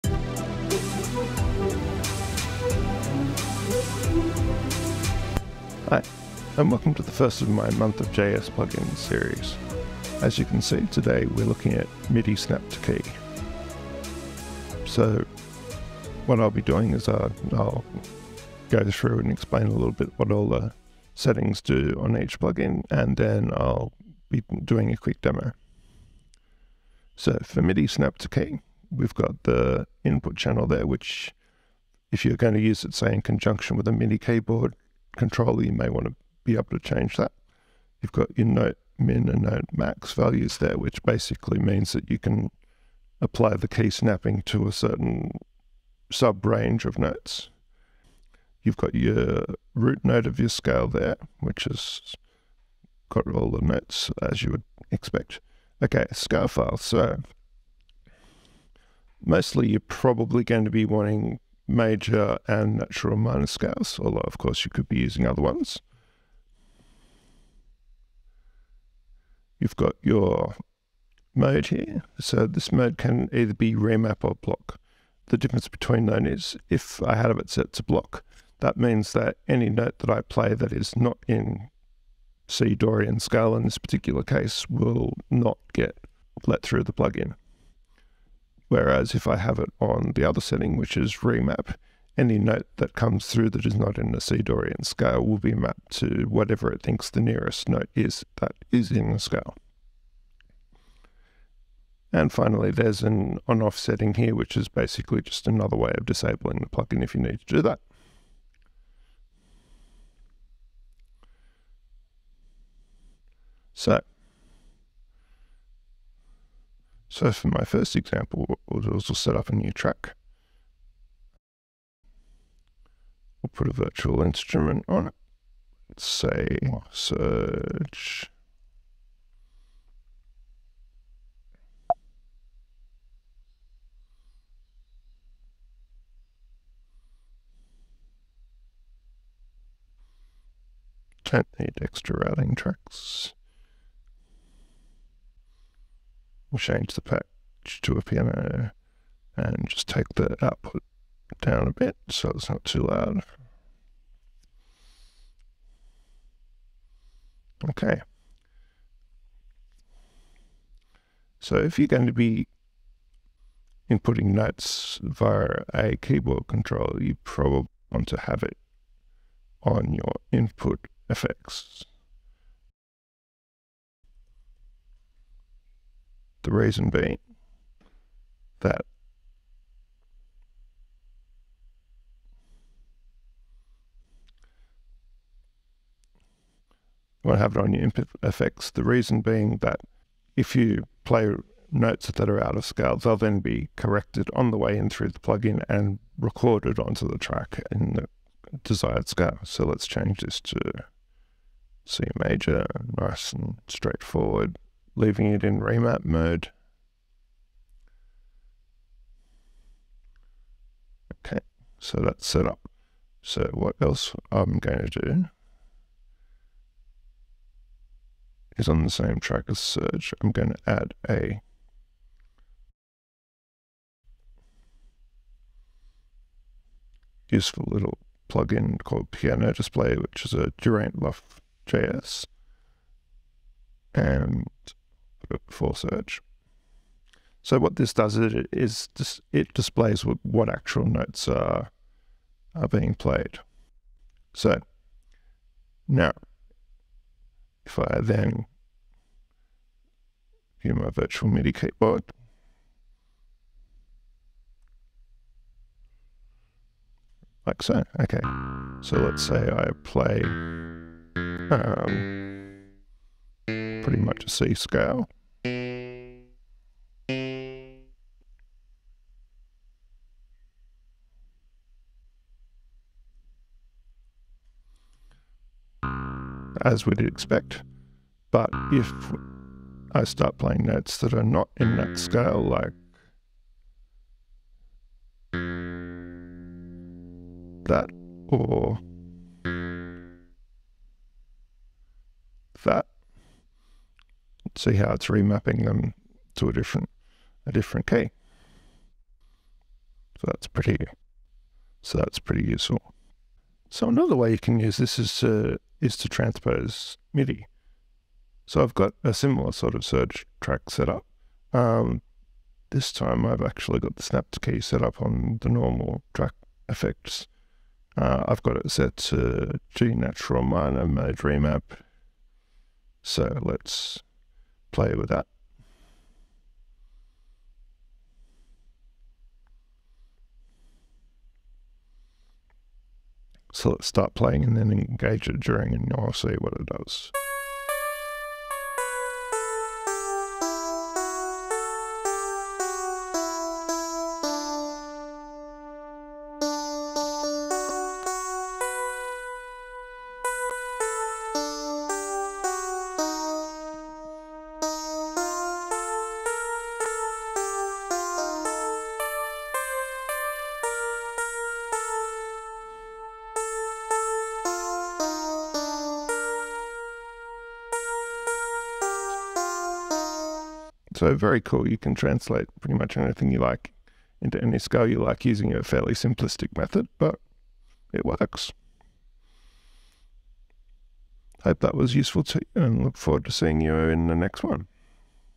Reaper Daily JS : Ep 1 - MIDI Snap To Key
The first episode of a new month long series covering a range of useful Reaper JS plugins!